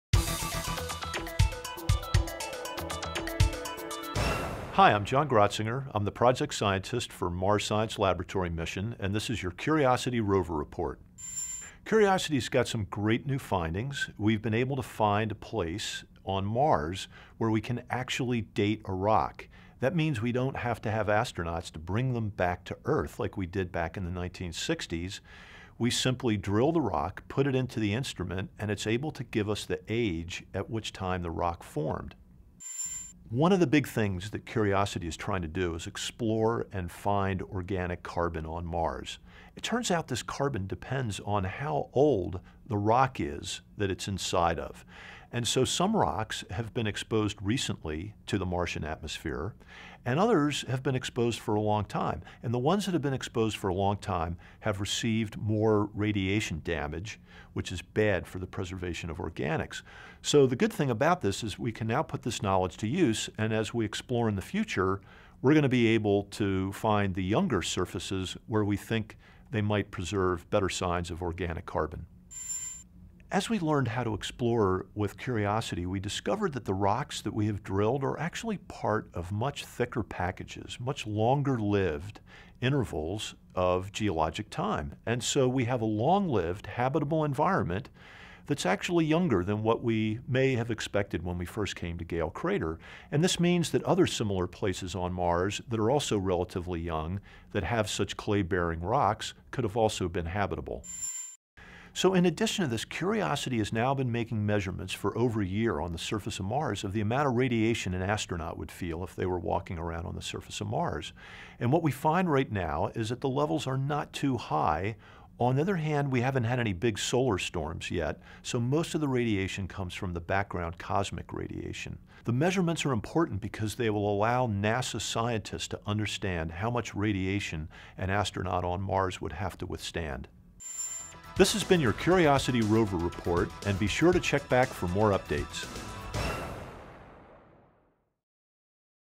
Curiosity: Mars Radiation Not Too High For Humans (but..) | Video
The Mars Science Laboratory radiation detector, in operation for over a year on the Red Planet, is showing levels that are manageable for human exploration but the planet has not been hit with any big solar storms during this time.